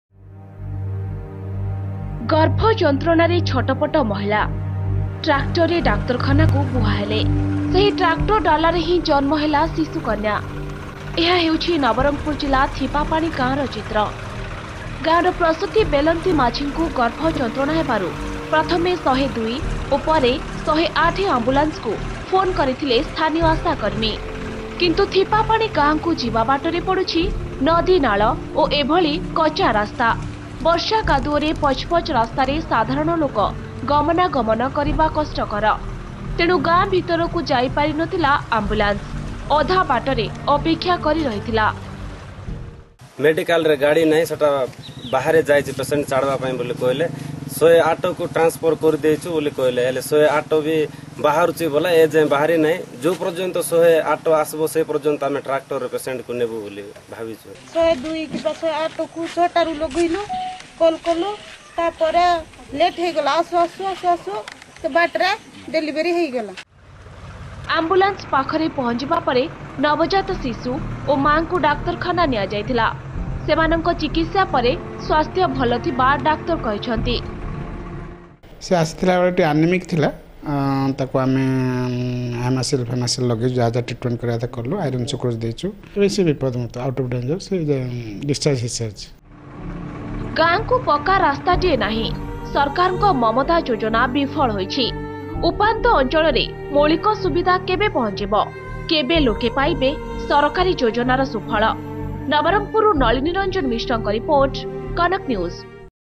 Lady Delivers Baby In Tractor As Ambulance Service Fails In Nabrangpur
A lady gave birth to a baby in a tractor as ambulance service failed in Nabrangpur